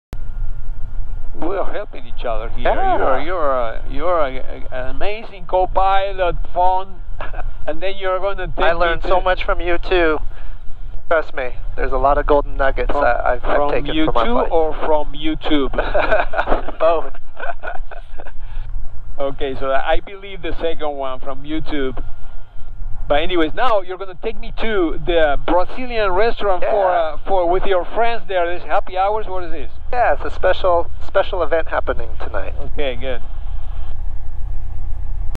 🤣Did You Say; from You Too, or from YouTube?
Embark on a round-trip adventure from Paine Field to Jefferson County and back—all through the immersive lens of Insta360! With a 360-degree perspective, you'll experience the flight as if you're in the cockpit. From pre-flight checks to landing back at base, you'll witness every aspect of this full-circle journey. Feel the power of the engine, the serenity of cruising altitude, and the focus required for a safe landing. Your virtual flight school awaits—buckle up and enjoy the ride! 🛫🌍🛬

Experience the thrill and precision of touch-and-go landings at Jefferson County Airport, all captured through the lens of Insta360! Step into the cockpit with me and gain a new perspective on flying. The 360° view lets you see every pivotal moment from engine roar to rubber meeting runway. Feel the exhilaration as we ascend, descend, and ascend again—each time honing our piloting skills. Perfect for aviation enthusiasts or anyone who's ever wondered what it's like to dance with the clouds. 🛫🌍

Except for those videos where a guest CFI is teaching something, my interaction in my videos is not intended to educate, train, instruct or teach you how to fly.